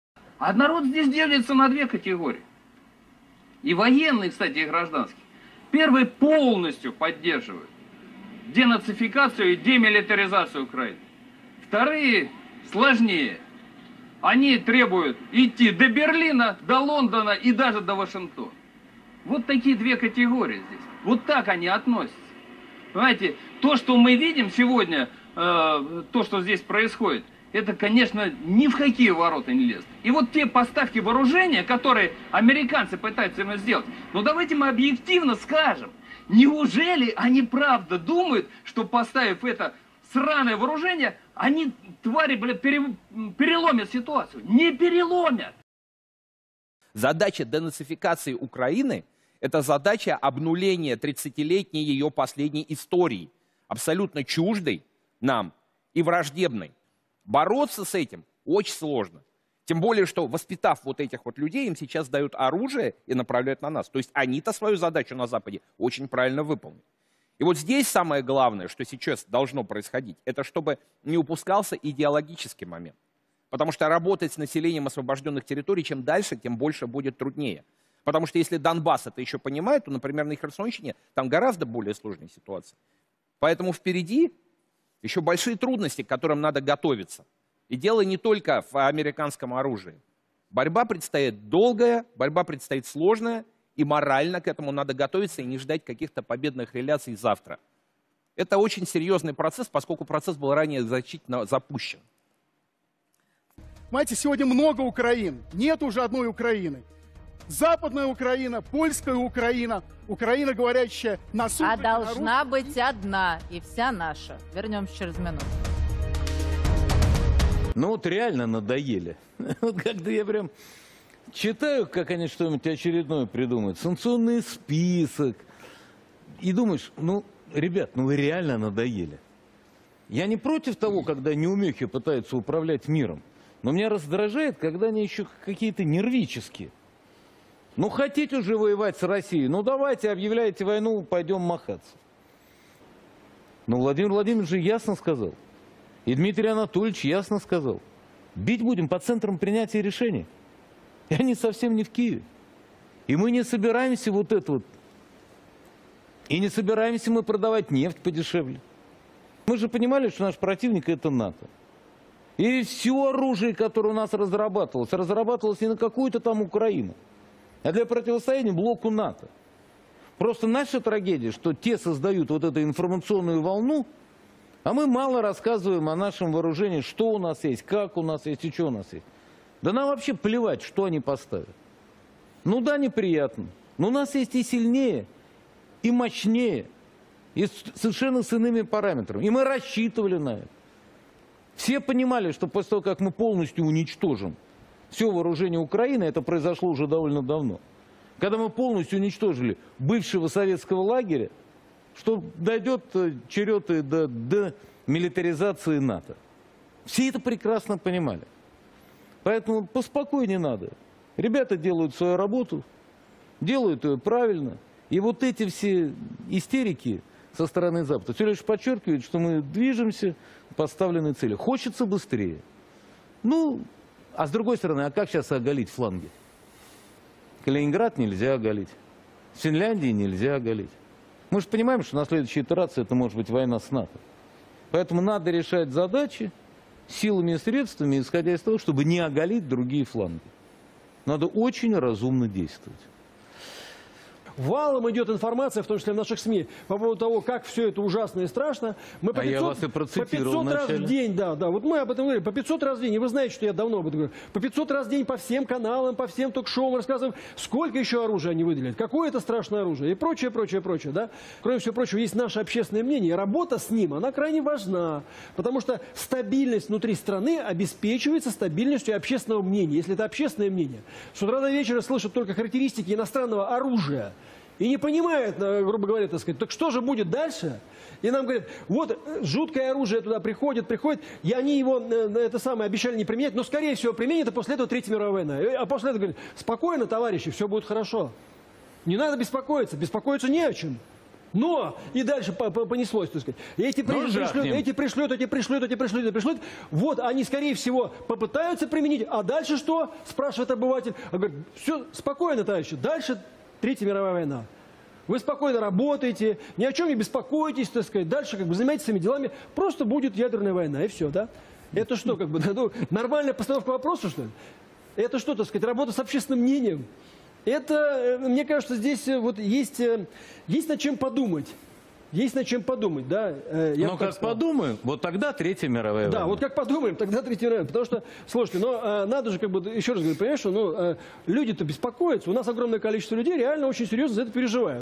Russian Propaganda Never Stops - You Can Not Make This Stuff Up
The propaganda machine of Russia never sleeps. It continues to develop the narrative of evil and the world must stand together to stop this madness.